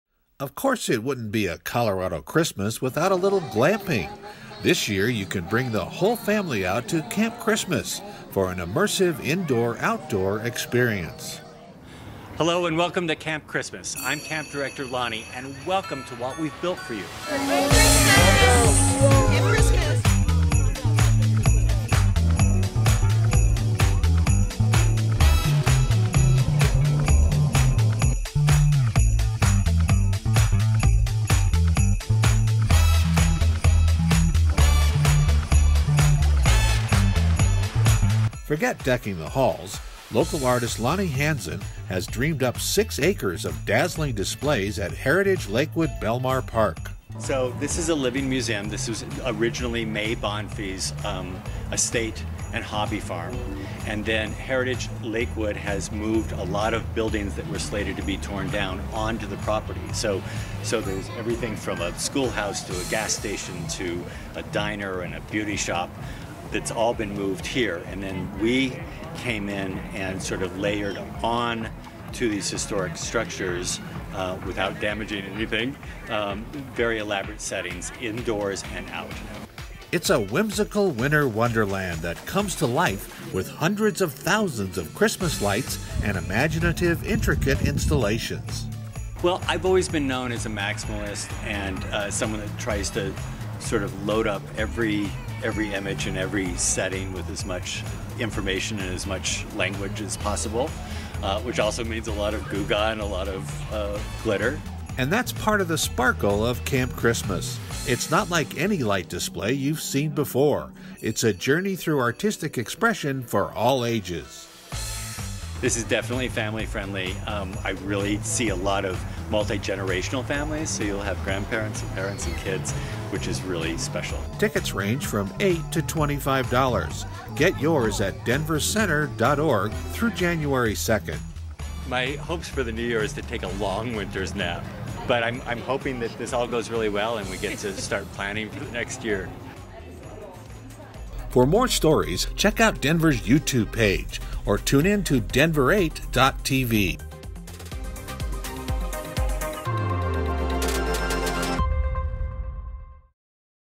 Your City Now: Camp Christmas
Lakewood's Belmar Park is alight in 6 acres of holiday joy and cheer. We talked to Lonnie Hanzon, the mind behind this winter wonderland, about his creation.